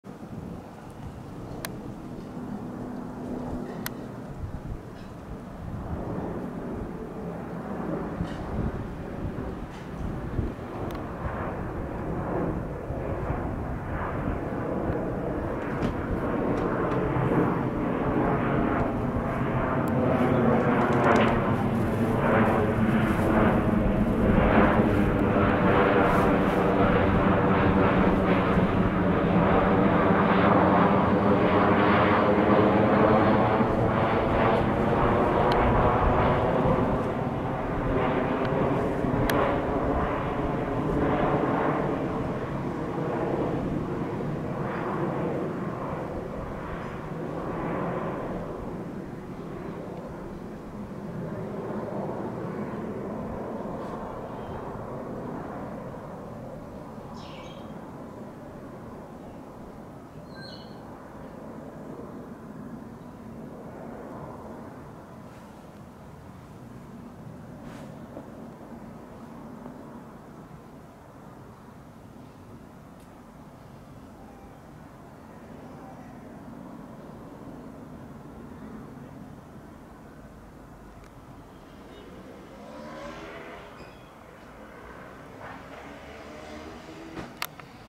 Lufthansa Flight 956 (Frankfurt to BHX)
Lufthansa Flight 956 from Frankfurt (Germany) to Birmingham Airport (England, UK). The aircraft is an Airbus A319-112 (reg: D-AIBJ and named Lorsch) is 5.0 years old and has been operating for Lufthansa since 2012.